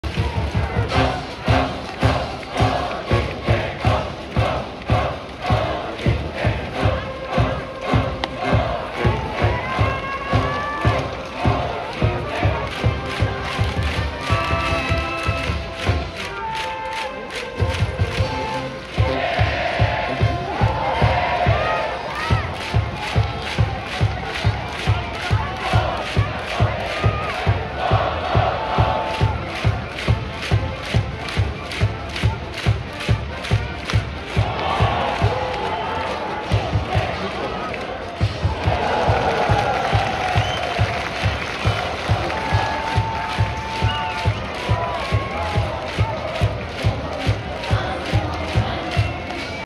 東日本国際大、森川大輝ナイスプレー
2024/06/15
早稲田大学対東日本国際大
東日本国際大、森川大輝、印出の
センターフライをナイスキャッチ